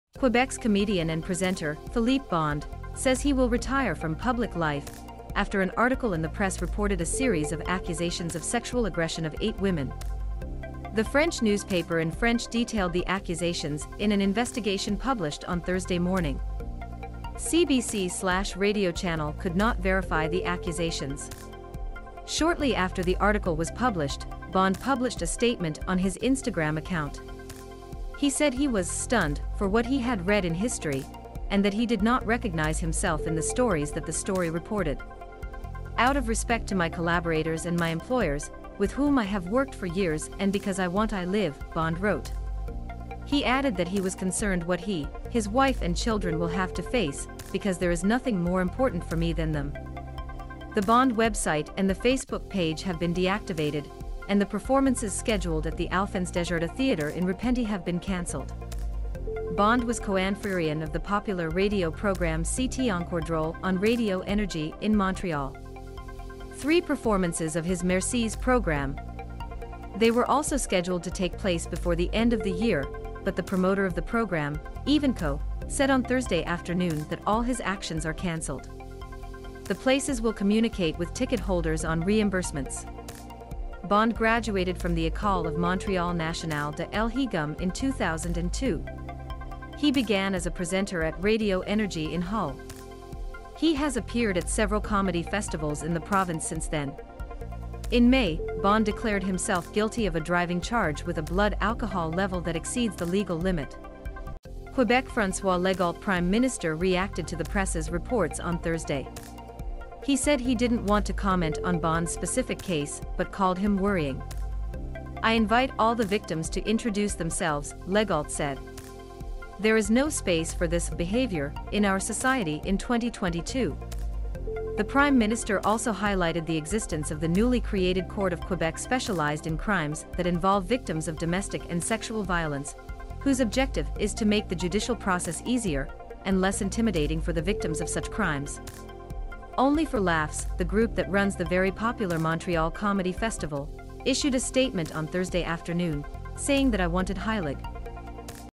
Quebec Philippe Bond comedian moves away from 'public life', after accusations of sexual assault
The comedian and animator of Radio du Québec, Philippe Bond, said that he would withdraw from "public life", after an article in the press reported a series of allegations of sexual assault by eight women.
Quebec's comedian and presenter, Philippe Bond, says he will retire from "public life", after an article in the Presse reported a series of accusations of sexual aggression of eight women. The French newspaper in French detailed the accusations in an investigation published on Thursday morning. CBC/Radio-channel could not verify the accusations. Shortly after the article was published, Bond published a statement on his Instagram account. He said he was "stunned" for what he had read in history and that he did not recognize himself in the stories that the story reported. "Out of respect to my collaborators and my employers, with whom I have worked for years and because I want I live, "Bond wrote. He added that he was concerned what he, his wife and children will have to face "because there is nothing more important for me than them." The Bond website and the Facebook page have been deactivated, and the performances scheduled at the Alphense-Desjardins theater in Repenty have been canceled. Bond was coanfrerion of the popular radio program c't'encore drôle on radio ennergie in Montreal. Three performances of his Merci's program! They were also scheduled to take place before the end of the year, but the promoter of the program, Evenko, said on Thursday afternoon that all his actions are canceled. The places will communicate with ticket holders on reimbursements. Bond graduated from the École of Montreal Nationale de L'Higum in 2002. He began as a presenter at Radio Ennergie in Hull. He has appeared at several comedy festivals in the province since then. In May, Bond declared himself guilty of a driving charge with a blood alcohol level that exceeds the legal limit. Quebec François Legault prime minister reacted to the Presse's reports on Thursday. He said he didn't want to comment on Bond's specific case, but called him worrying. "I invite all the victims to introduce themselves," Legault said. "There is no space for this [behavior] in our society in 2022." The Prime Minister also highlighted the existence of the newly created Court of Quebec specialized in crimes that involve victims of domestic and sexual violence, whose objective is to make the judicial process easier and less intimidating for the victims of such crimes. Only for laughs, the group that runs the very popular Montreal Comedy Festival, issued a statement on Thursday afternoon, saying that I wanted Highlig